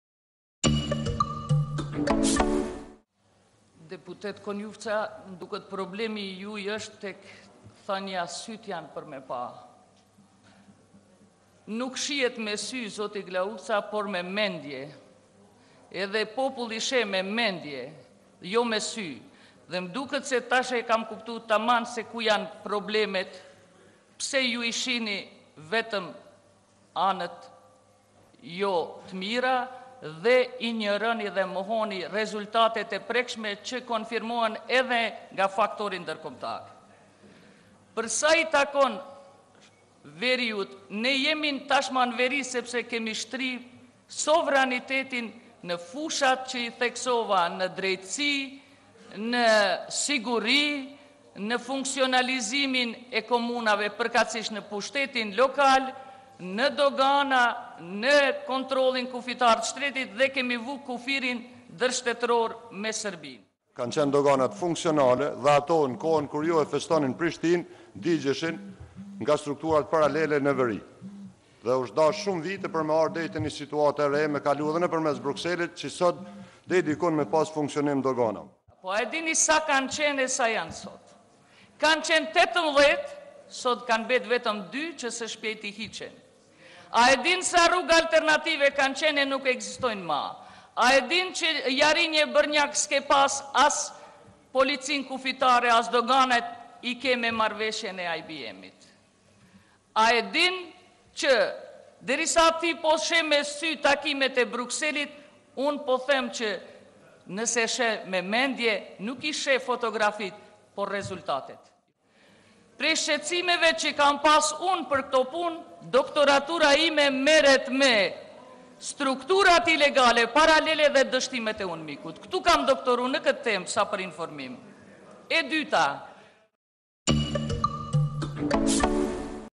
Tahiri numëron sukseset, opozita ia kujton 'fusnotën'
Seanca e Kuvendit të Kosovës të enjten ka filluar me replika të shumta mes zëvendëskryeministres Edita Tahiri me deputetin e Vetëvendosjes, Glauk Konjufca dhe atë të AAK-së Daut Haradinaj, rreth bisedimeve dhe integrimit të veriut të Mitrovicës.